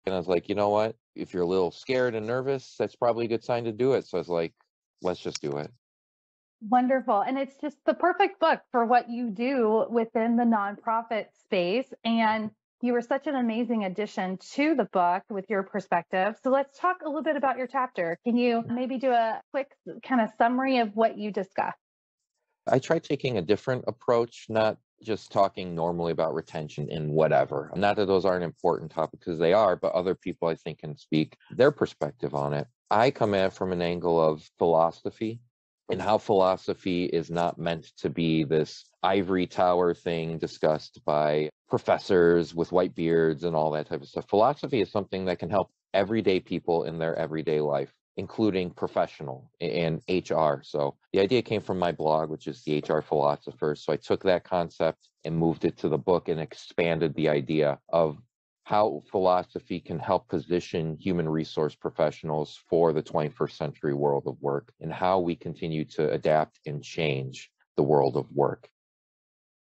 Book Smarts Business Podcast - Paul Lalonde
Can philosophy help HR professionals in this ever-changing 21st century workplace? Paul explains his thoughts on how philosophy is really meant for everyday people and their everyday lives. He also shares his experiences working at a not-for-profit and what it feels like writing in the Amazon best-selling book, People Fusion.

Bio: Paul LaLonde
Paul A. LaLonde, SHRM-CP, is a deliberate HR professional who believes philosophy is a difference-making pursuit in life as well as in business. Learning, teaching, and being of service to others are his purpose. Currently, Paul serves as the V.P. of People & Culture for CEDA of Cook County, Inc. in Chicago, serves as an adjunct professor at Joliet Junior College, and manages his consulting and coaching business, HR Logic. Paul is an alumnus of the SHRM National Blog Squad, named to Social Micole’s Most Inclusive HR Influencer List™ in 2020 and 2021, and a recipient of the 2021 HR Unite! Community Award, and recipient of a 2021 Human Resources Today MVP Award for his blog HR Philosopher. He is a proud husband, father, disobedient cat owner, Star Wars nerd, and metalhead. Connect with him on LinkedIn and Twitter at @HRPaul49.